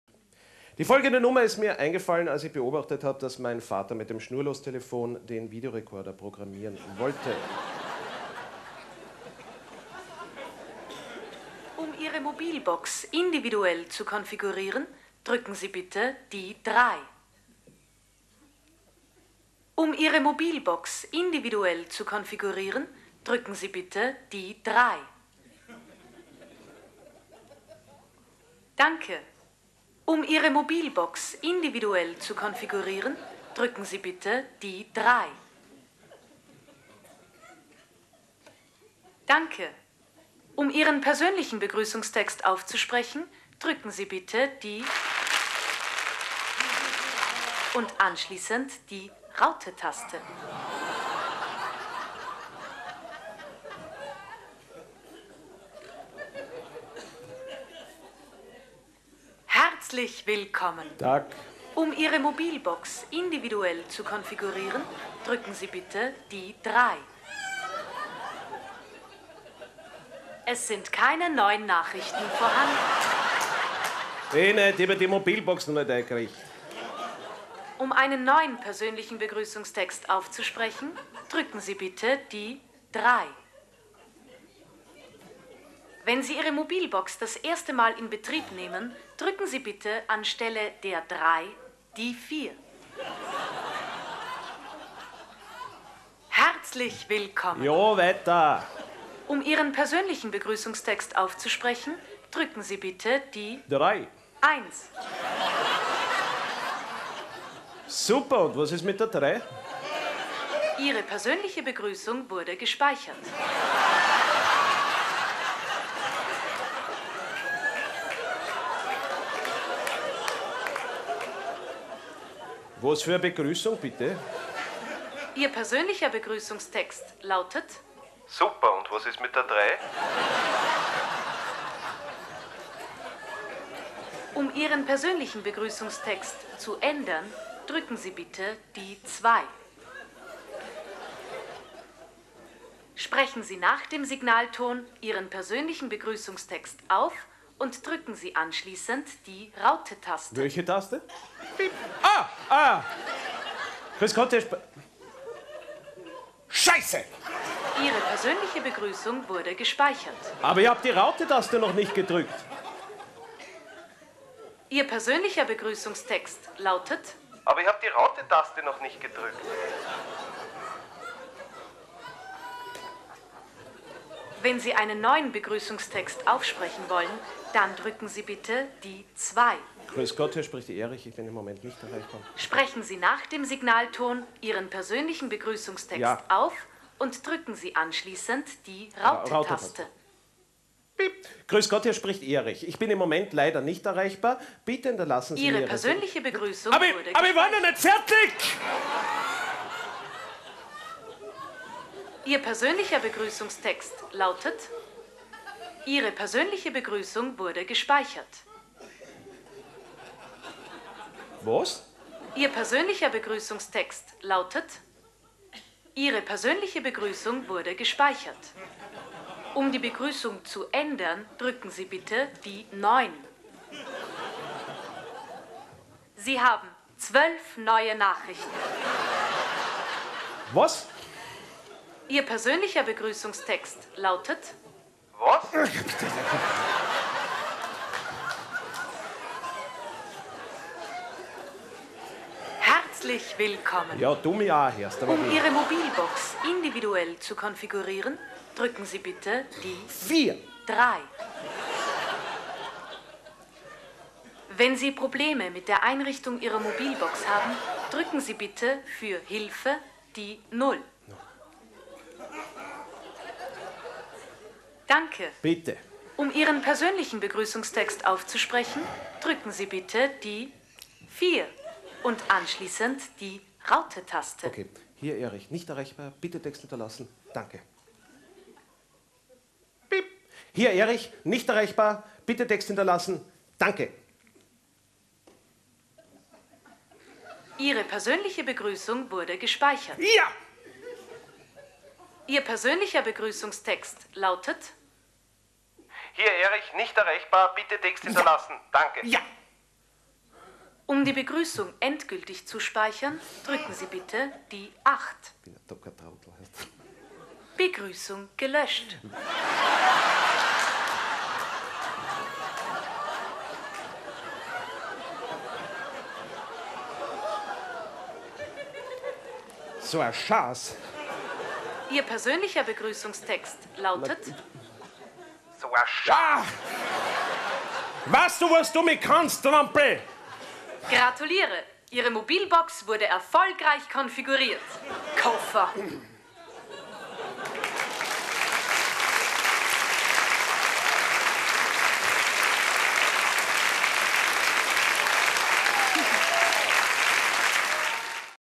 HEILBUTT & ROSEN - "Die neue Mobilbox"
Für diese Kabarett Gruppe habe ich fast alle Programme inszeniert und auch sehr viele Texte mitgeschrieben. Das ist einer der ersten Sketche davon.

"Heilbutt & Rosen" war eine österreichische Kabarettgruppe, die 1992 von den Studienkollegen Helmuth Vavra und Berthold Foeger gegründet wurde¹². Trotz des Namens ist Heilbutt & Rosen keine Duo, sondern eine Kabarettgruppe, die in unterschiedlichen Besetzungen in ganz Österreich auftritt¹²³.

Ab 2004 war Helmuth Vavra alleiniger Produzent der Kabarettprogramme und Shows¹. Die Gruppe hat sich in ihren Anfängen überwiegend der Texte von Monty Python und Loriot angenommen, daher auch der ungewöhnliche Gruppennamen Heilbutt (für Monty Python) & Rosen (für Loriot)¹³. Mit ihren Alltagssatiren und Charakteren sind Heilbutt & Rosen diesen Wurzeln ihres Humors treu geblieben¹.

Zu den weiteren Mitgliedern gehören unter anderem Theresia Haiger, Reinhard Nowak, Sigrid Spörk, Verena Scheitz und Markus Oezelt¹². Die Gruppe behandelt in satirischer Weise Themen des Alltags, insbesondere die Beziehung zwischen den Geschlechtern²⁴.